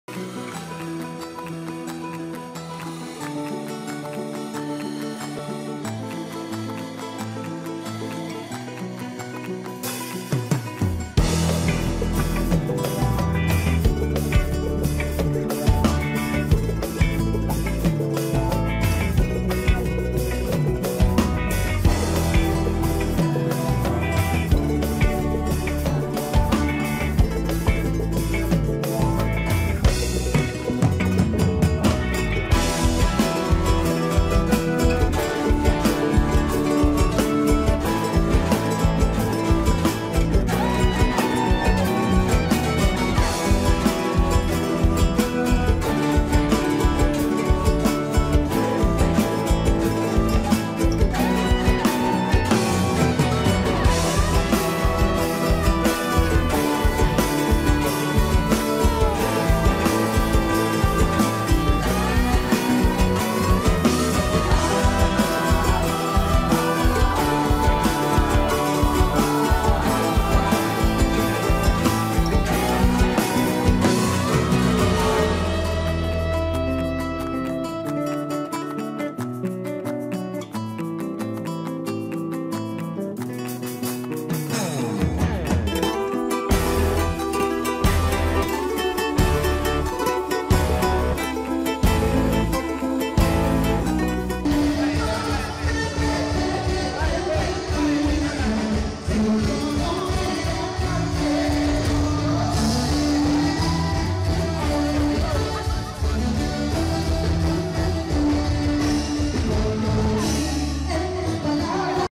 Daniela Prado y banda en Festival Lima Vive (reel)
Daniela Prado y banda participaron en el Festival Lima Vive junto a Amaranta, Pelo  D’ Ambrosio, Max Castro y Mac Salvador